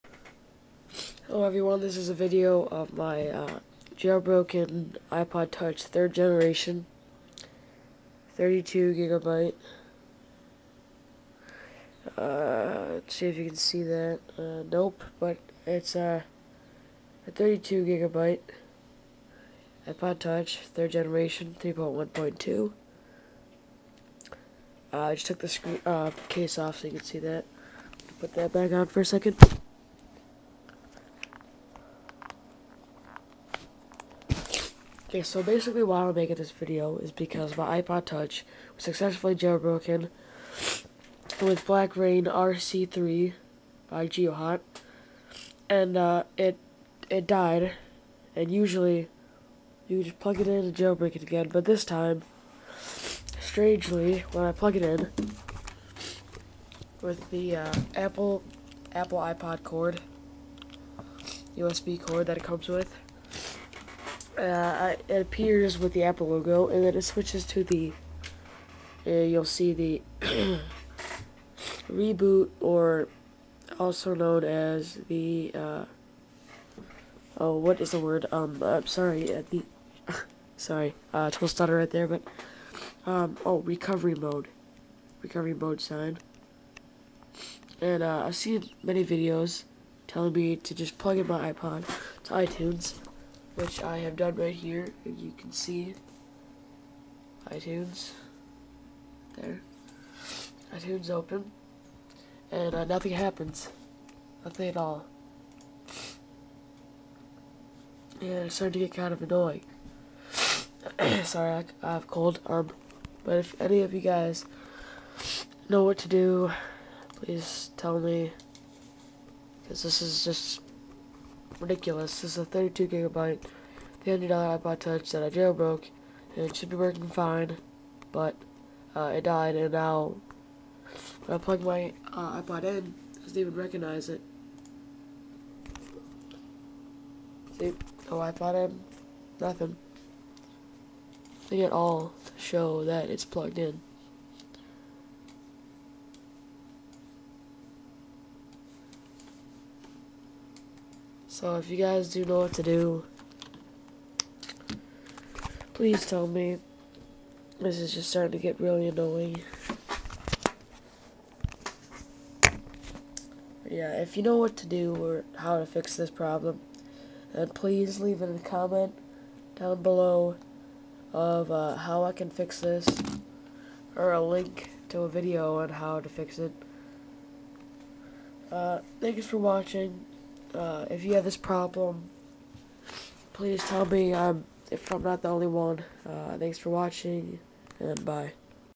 How do I reboot my tethered iPod Touch 3g???
***PLEASE WATCH*** ok so I sucessfully jailbroke my iPod touch 3g version 3.1.2. And it died. I`ve watched many videos on how to reboot it. I should just have to plug it into my computer and iTunes will open up sayig "An iPod touch has been pluged in that has to be recovered" BUT when I plug my iPod into my computer, my computer doesn`t even recognize it. Please help